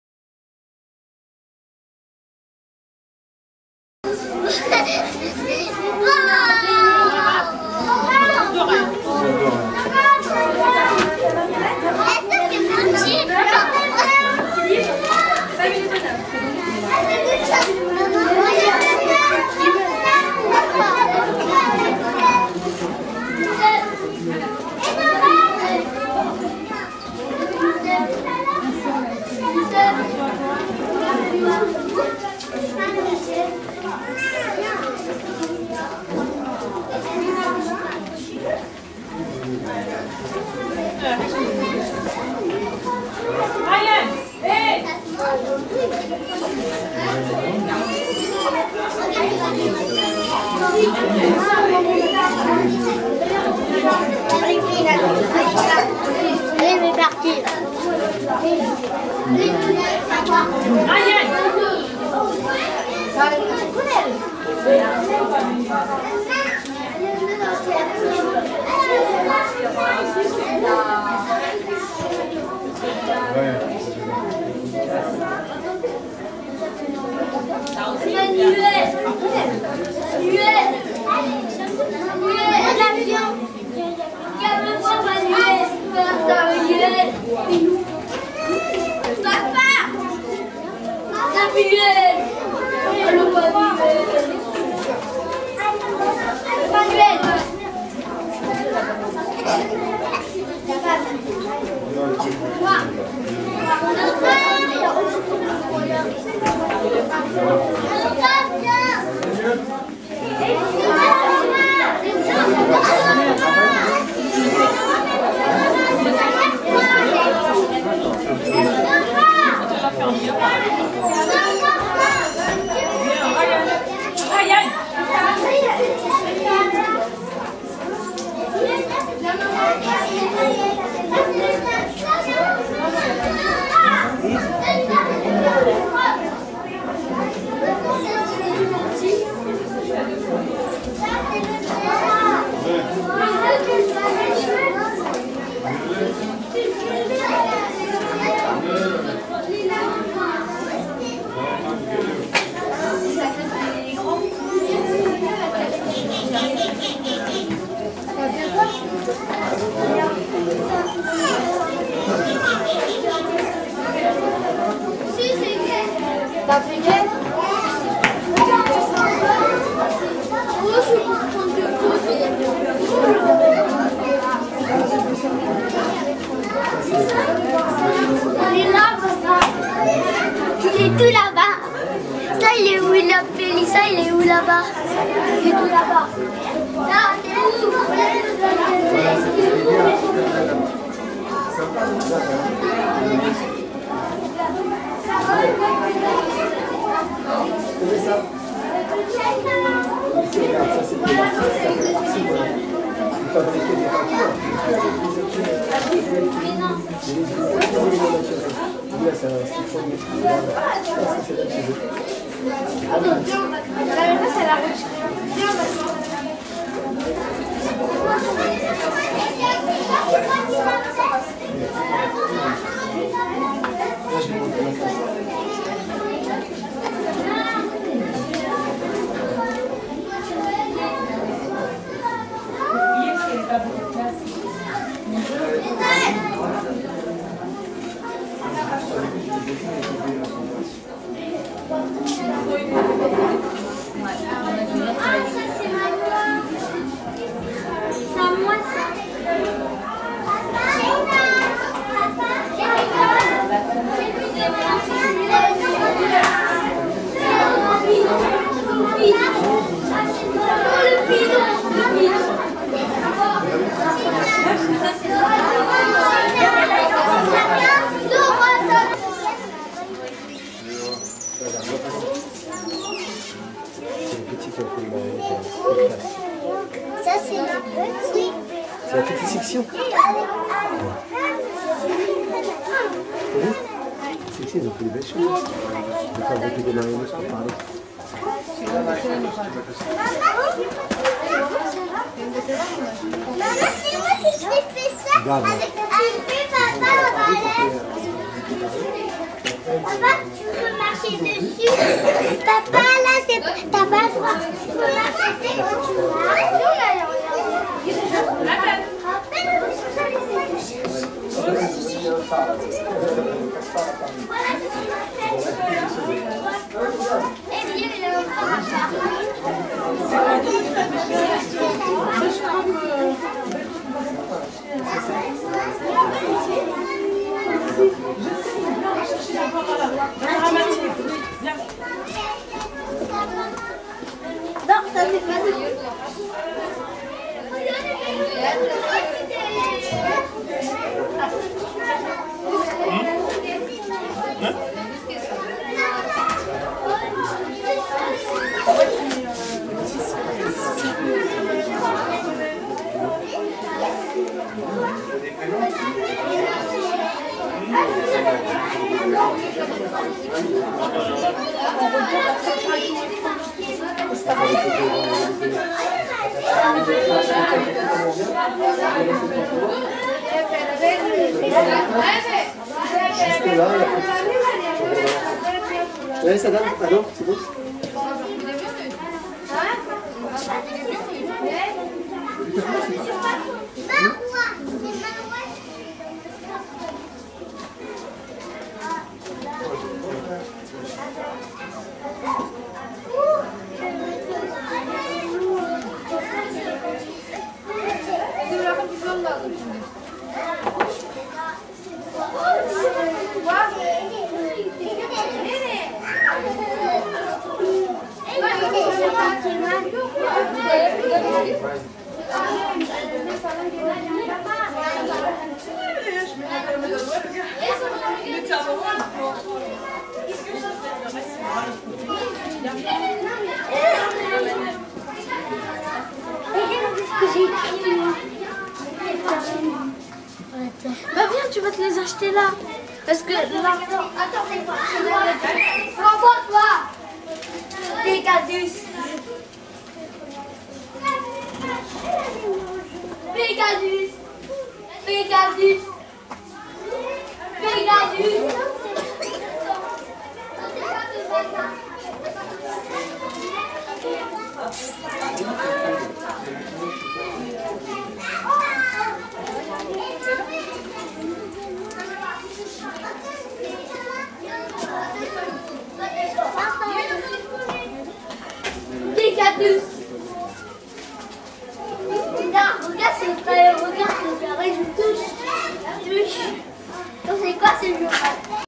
Fête à l'école Savigny à Aulnay-sous-Bois en juin 2012
Fête à l'école Savigny à Aulnay-sous-Bois en juin 2012. Vidéo pour le site d'information